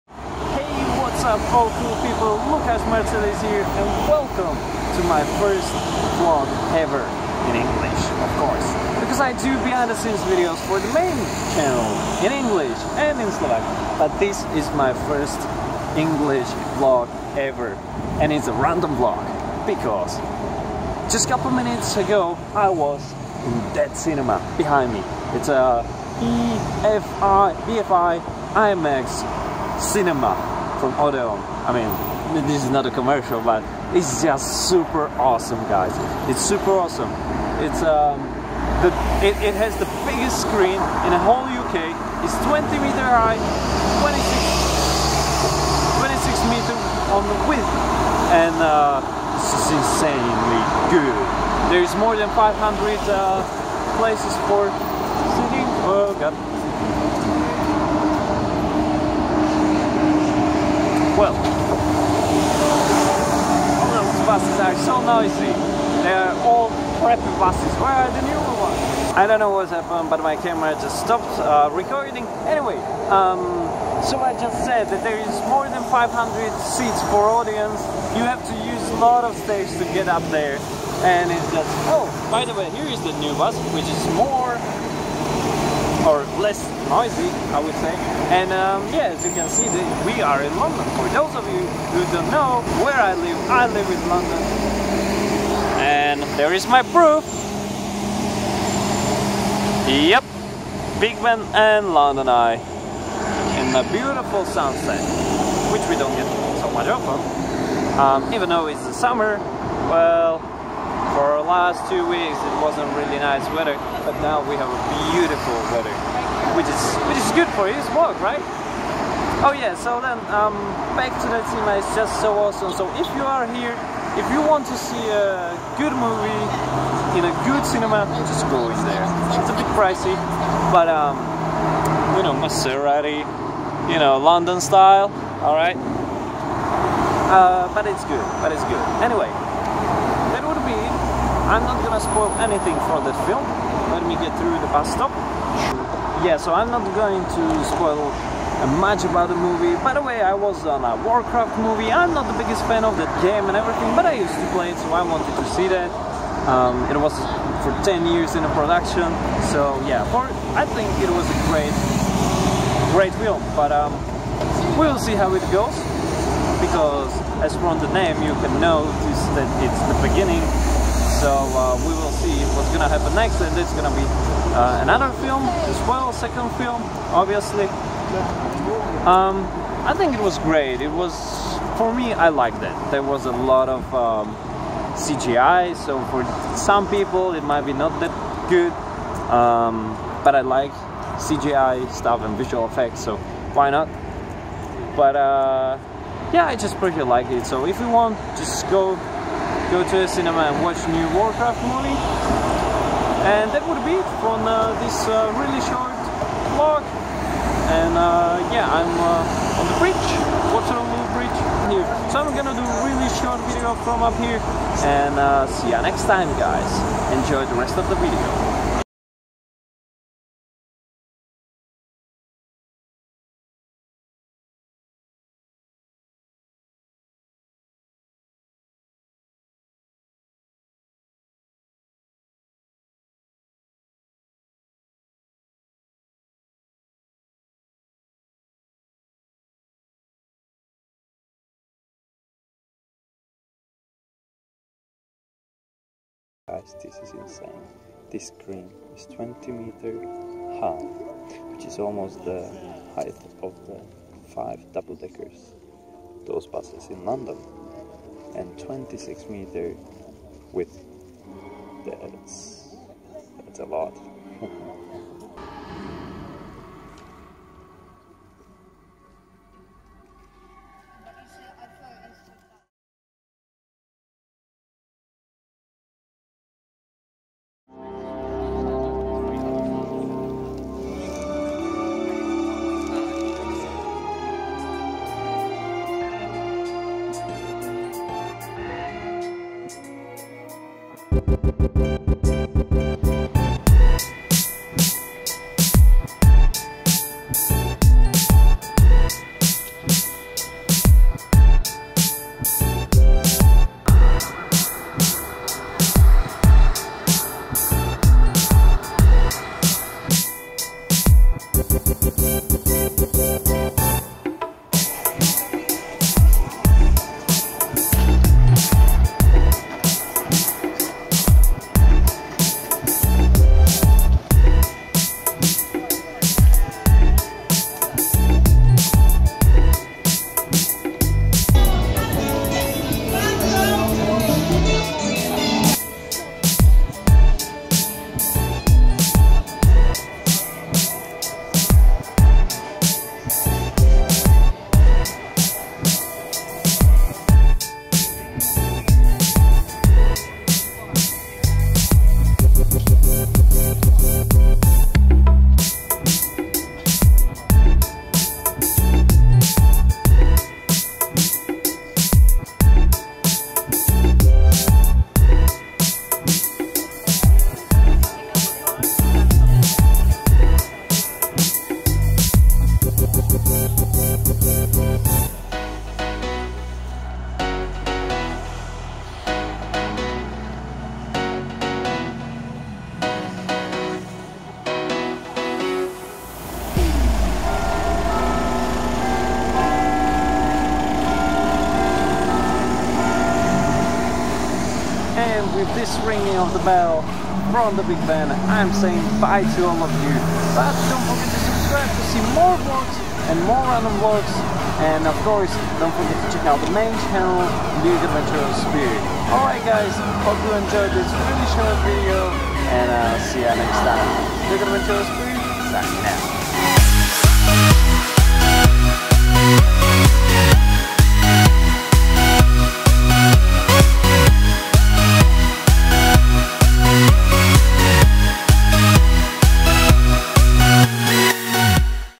THE LARGEST IMAX CINEMA SCREEN IN BRITAIN !!! IT'S HUGE | Random vlog | Eng
Do you know how does it feel to see a film on the biggest IMAX screen in Britain?

It feels AMAZING. Rich colors, sharp details, great 3D and powerful 3D sound.

This BFI IMAX cinema has the largest IMAX screen in Britain with dimensions of over 20 m. high and over 26 m. wide.

That is huge.  Hope you enjoy the video.  :)